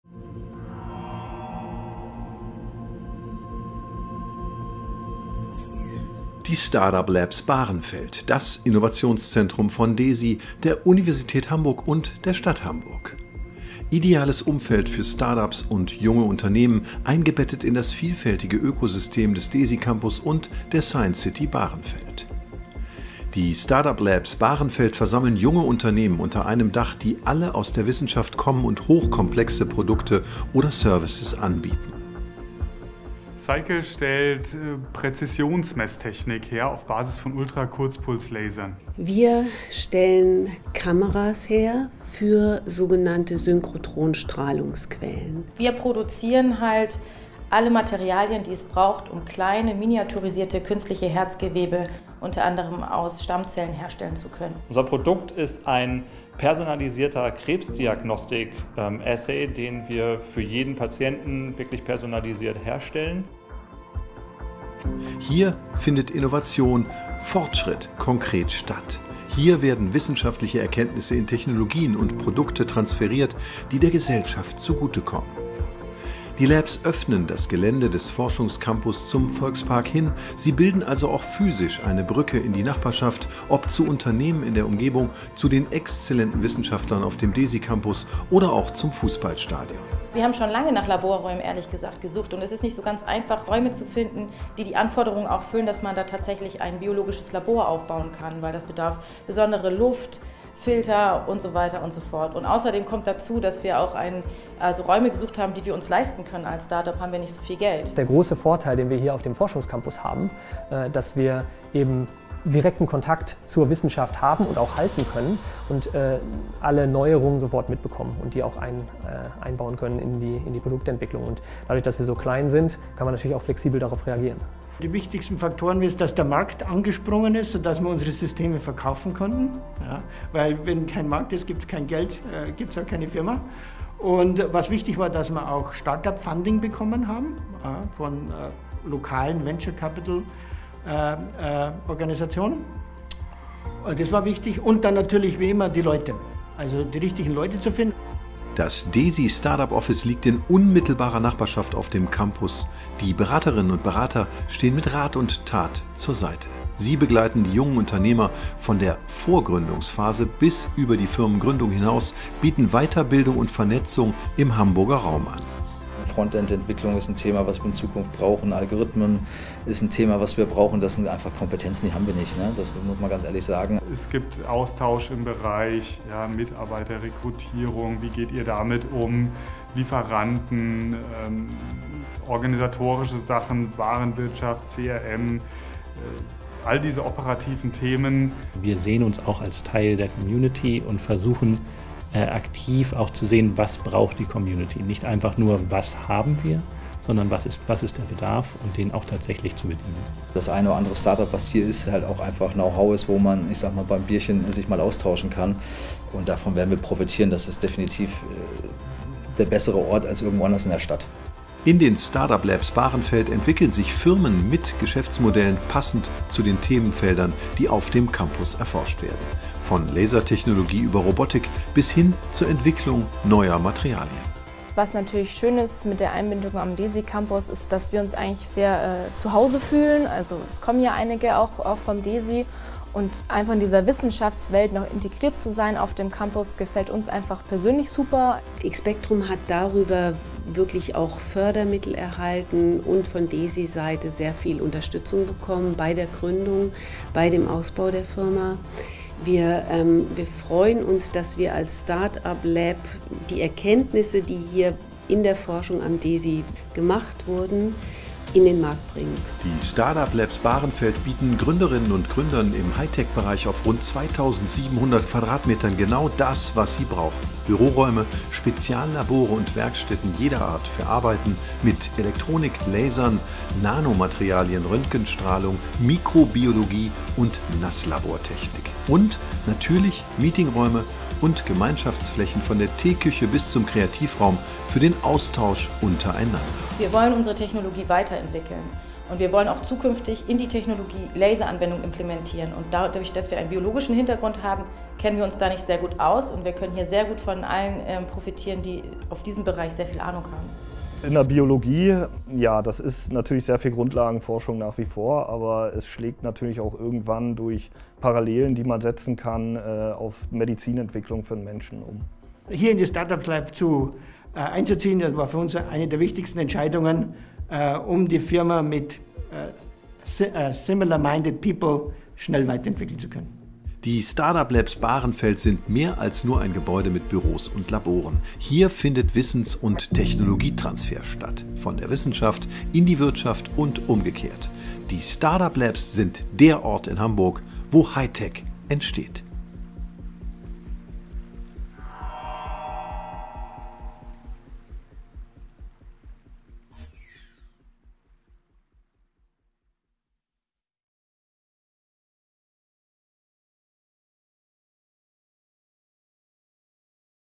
Start-up Labs Bahrenfeld
Die Start-up Labs Bahrenfeld, das gemeinsame Innovationszentrum von DESY, der Stadt Hamburg und den Universität Hamburg: Der Ort für junge, innovative Unternehmen mit spannenden Themen aus dem Deeptech-Bereich auf dem Forschungscampus in Hamburg-Bahrenfeld. Das Gebäude bietet den Start-ups auf 2.700 Quadratmetern Büros, Labore und Werkstätten genauso wie Meeting- und gemeinsame Aufenthaltsräume. Die Start-up Labs Bahrenfeld sind ein weiterer Baustein, wo die Science City Hamburg-Bahrenfeld greifbar wird.